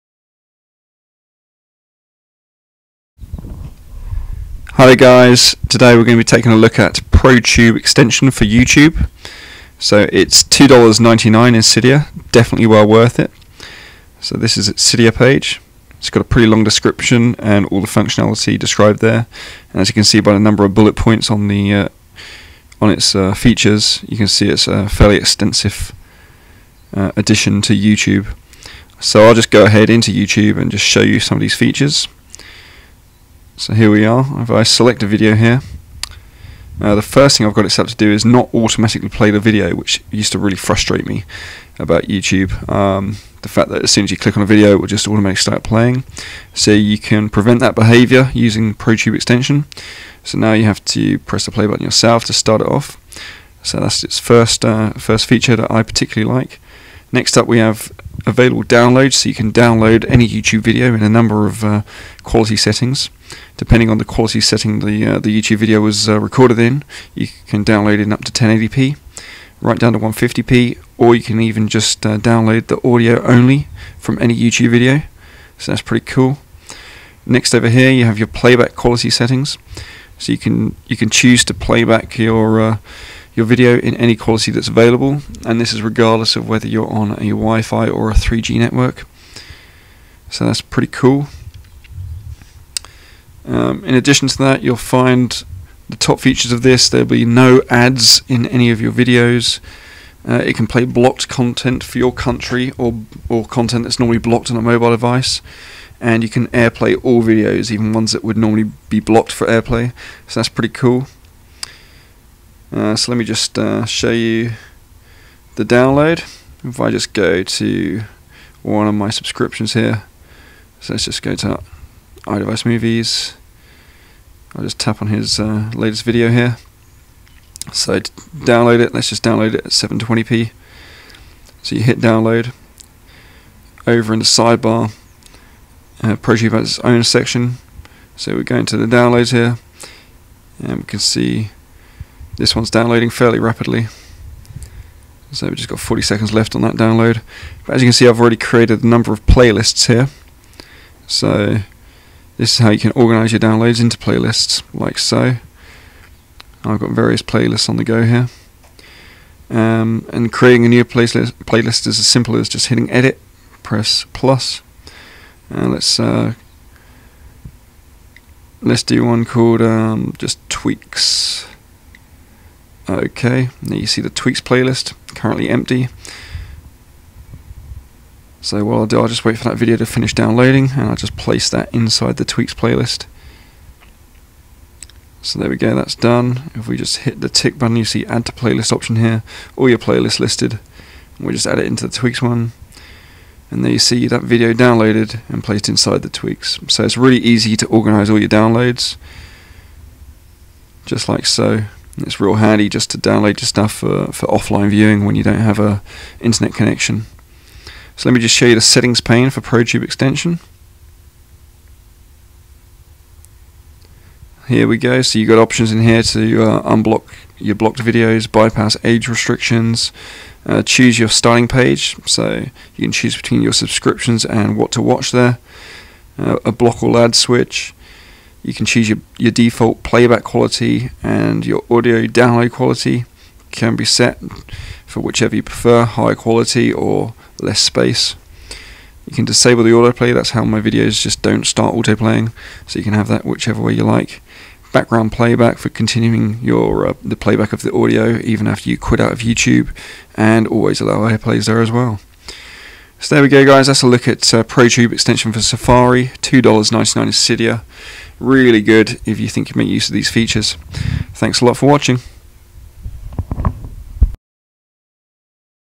ProTube Extension for YouTube Review
I take a look at ProTube Extension for YouTube. Increases the functionality of YouTube on iPhone via a Cydia tweak.